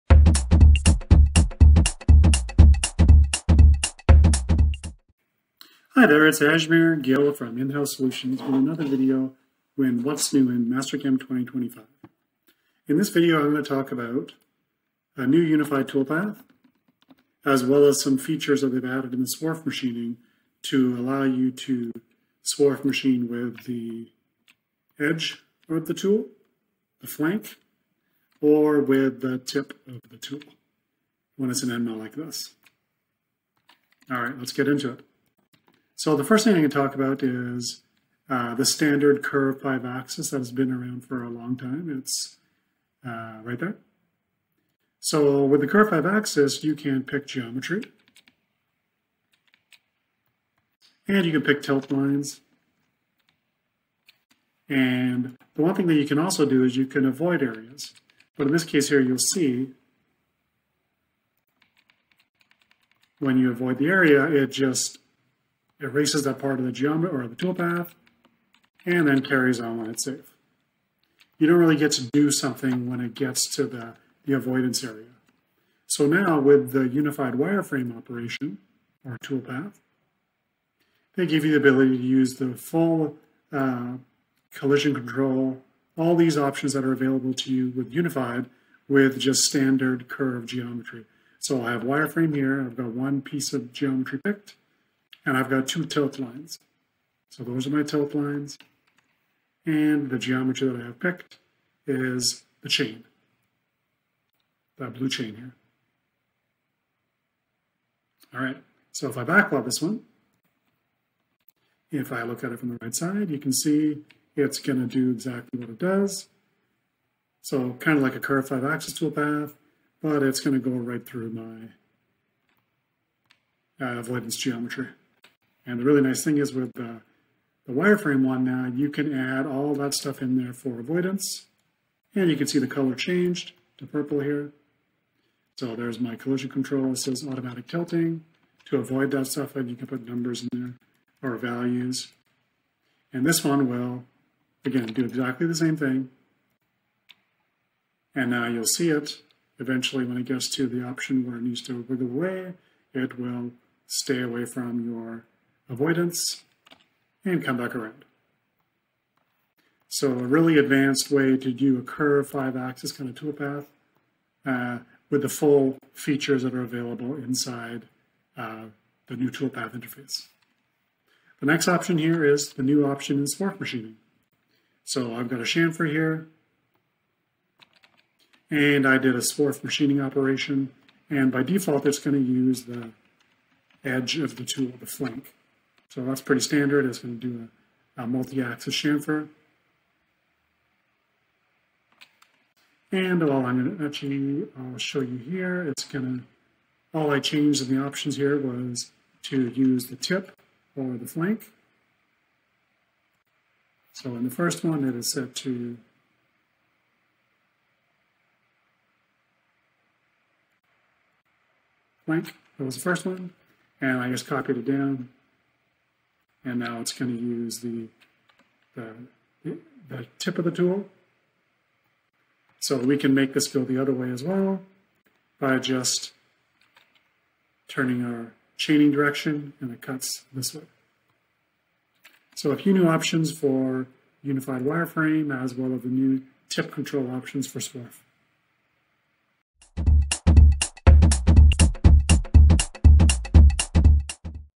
New Multiaxis Toolpath Updates in Mastercam 2025
Follow along as Ajmer goes over the new unified toolpath as well as some features that have been added to swarf machining to allow you to swarf machine with the edge, the flank or the tip of the tool.